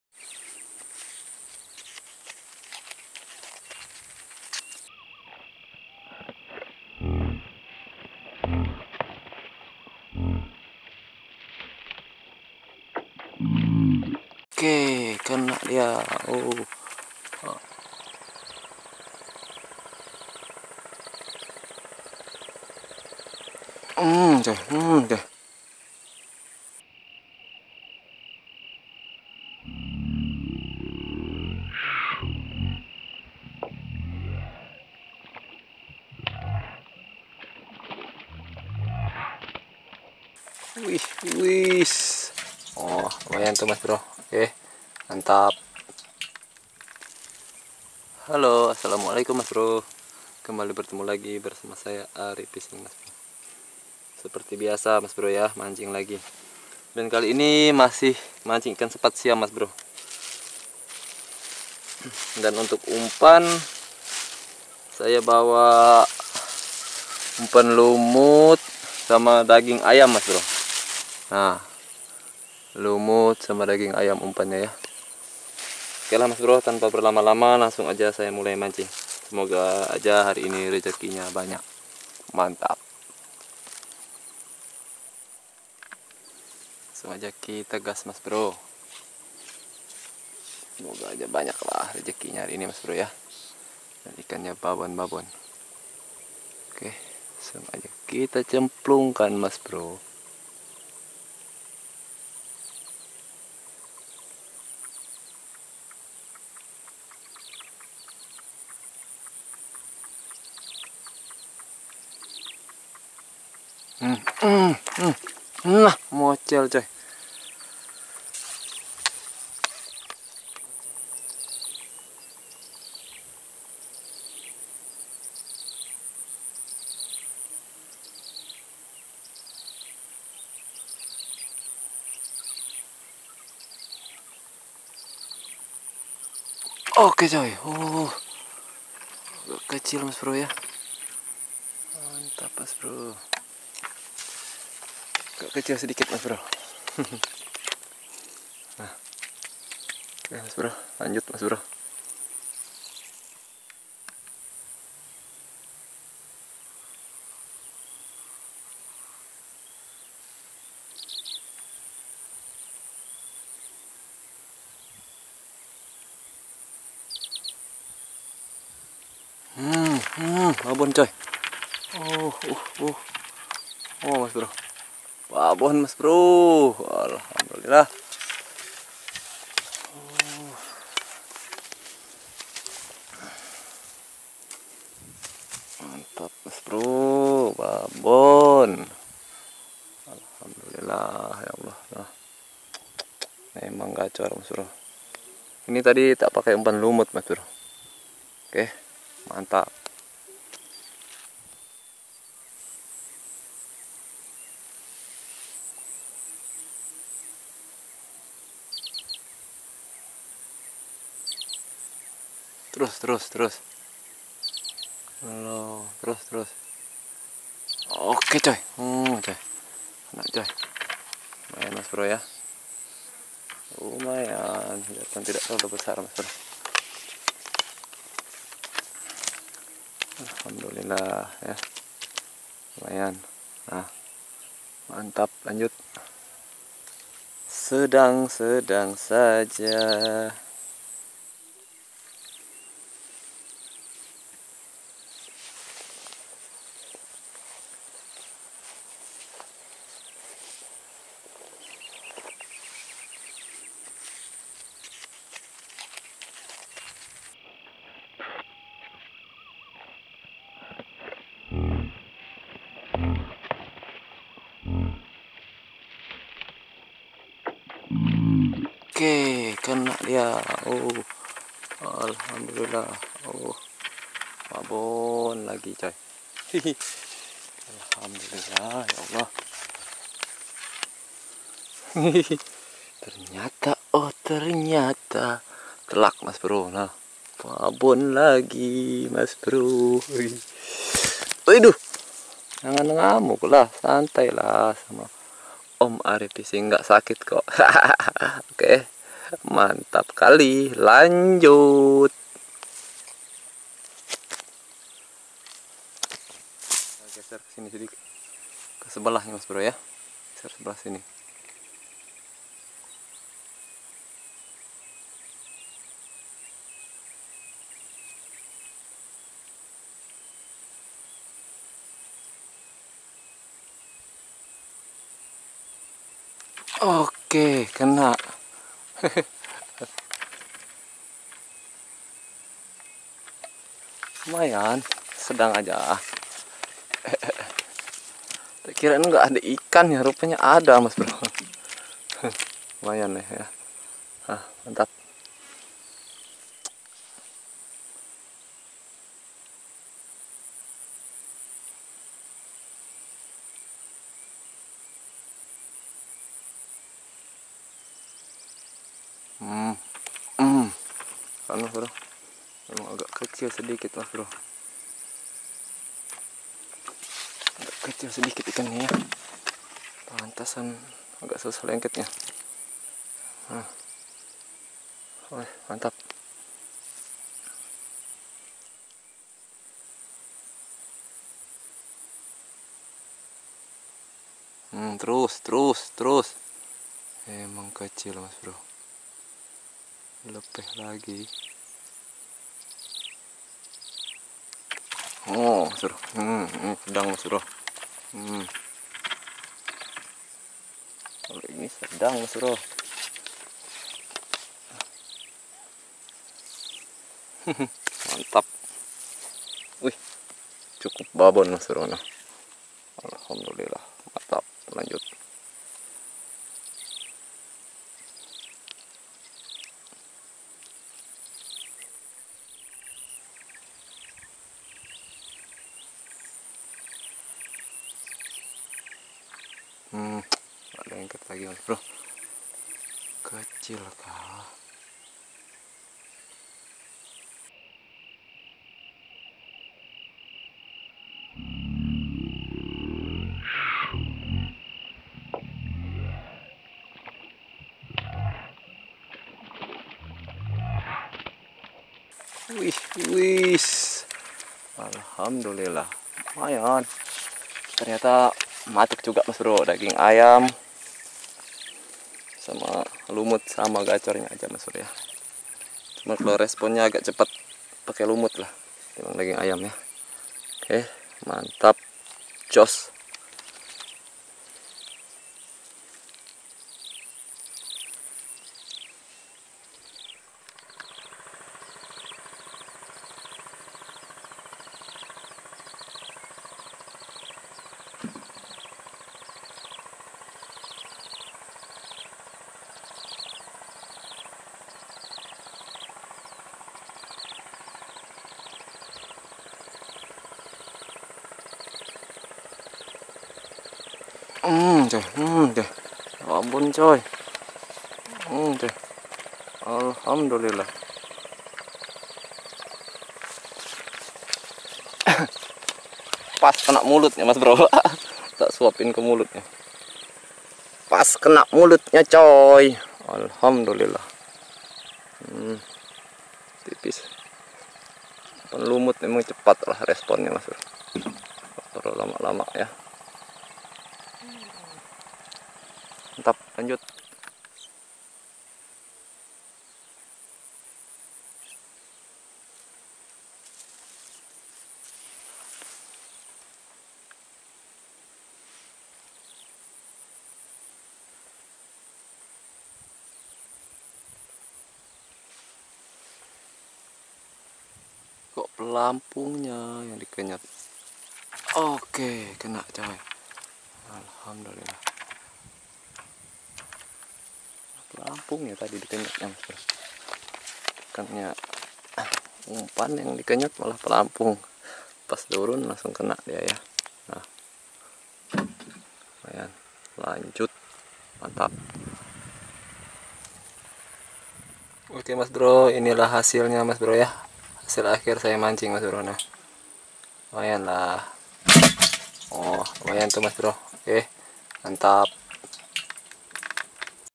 ASLI GACOR..!! TAK SALAH PILIH UMPAN SEPAT BABON TAK BERKUTIK DI BUAT NYA,MANCING SEPAT SIAM
assalamualaikum mas bro jumpa lagi bersama saya Arie fishing
seperti biasa kali ini mancing sepat Siam lagi dan spot nya di rawa dan kali ini saya menggunakan jenis umpan yang berbeda sama-sama ampuh tahlukan sepat Siam babon ok seperti apa keseruan nya,silahkan mas bro simak video nya sampai selesai

hasil:banyak dapat ikan sepat Siam babon

(set peralatan)
pancing tegek panjang 450cm
mata kail no 03 carbon
pelampung pancing bekas kabel antena TV

camera: handycam Sony cx 405
apk editing kinemaster
apk thumbnail piksellab

lokasi spot:rawa Kalimantan tengah kabupaten Lamandau, Nanga bulik

umpan:lumut dan daging ayam